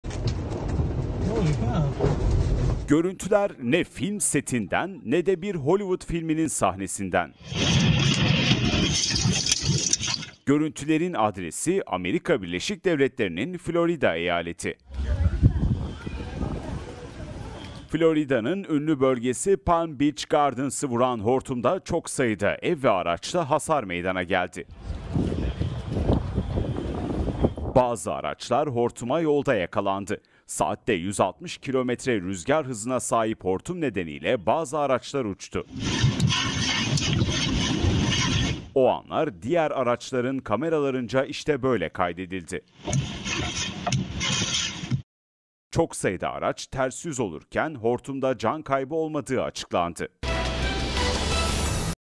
ABD’de Hollywood Filmlerini Aratmayan Görüntüler! Hortum Arabaları Havaya Uçurdu! - TGRT Haber
ABD’nin Florida eyaleti hortum felaketiyle sarsıldı. Araçlar uçtu, evlerde büyük hasarlar oluştu. Hollywood filmlerini aratmayan anlar araç kameralarına yansıdı.